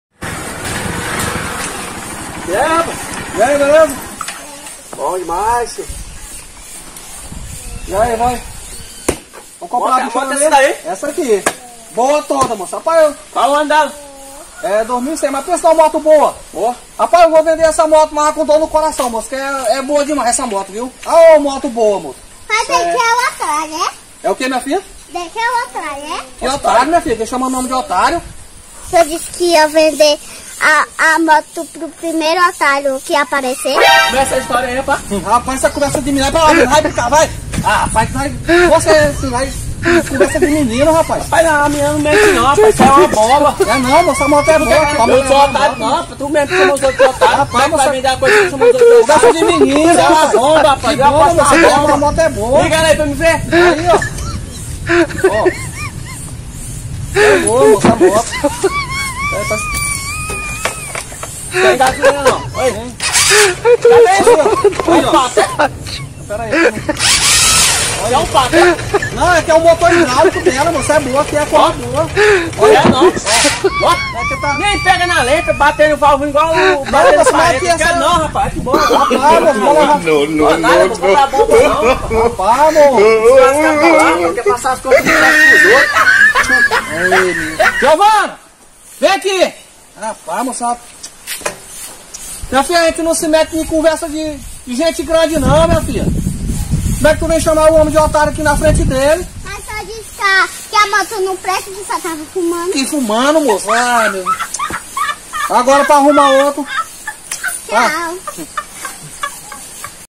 o golpe tá aí!
Menina estragou a venda do pai

vídeo engraçado